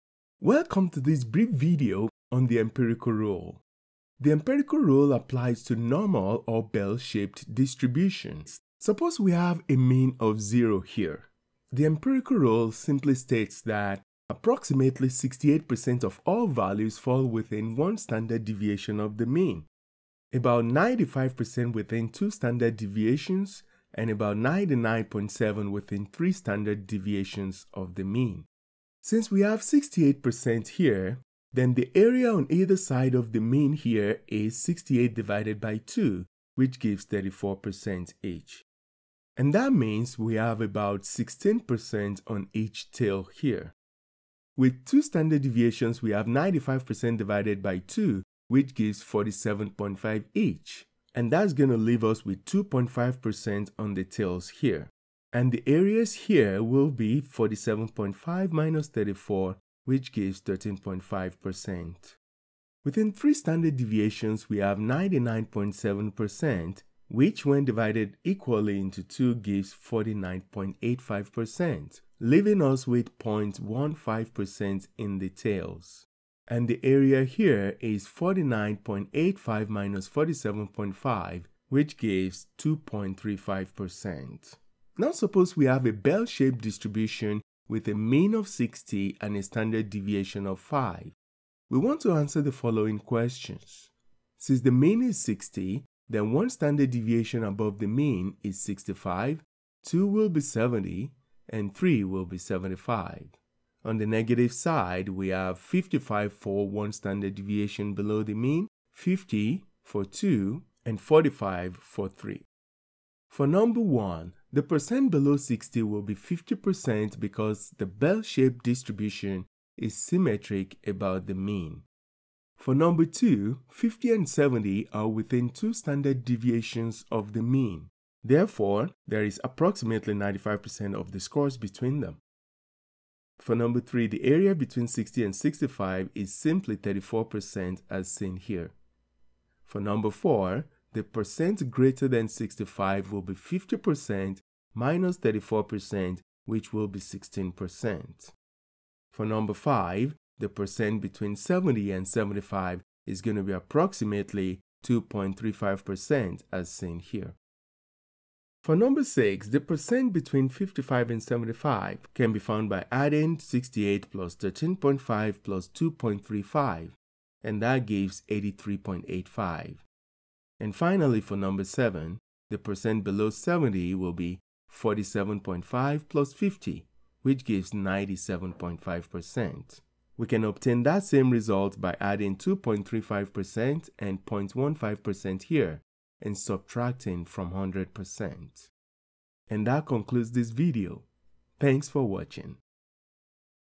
Empirical Rule (68-95-99.7) for Normal Distributions
This video describes the empirical rule and shows its application given the mean and standard deviation of a bell-shaped distribution.